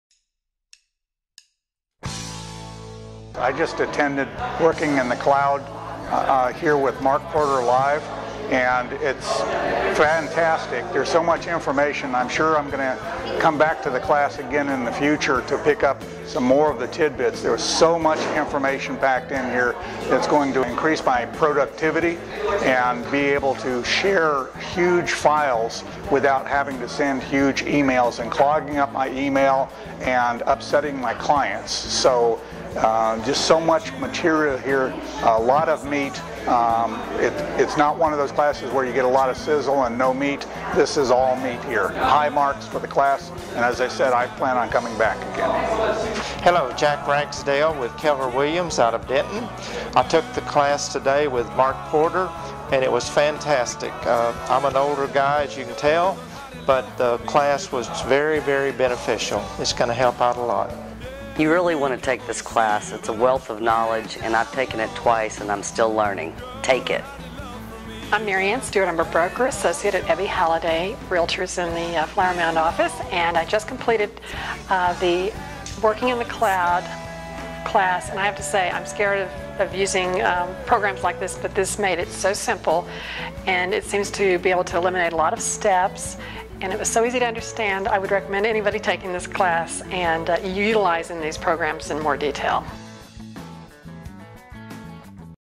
Working in the Cloud Testimonials | Mark Porter Live
If you're not using cloud computing in your business, you're not
working as efficiently as you could be! Check out what real estate
agents in the Dallas / Ft. Worth area had to say about the benefits of
cloud computing for real estate.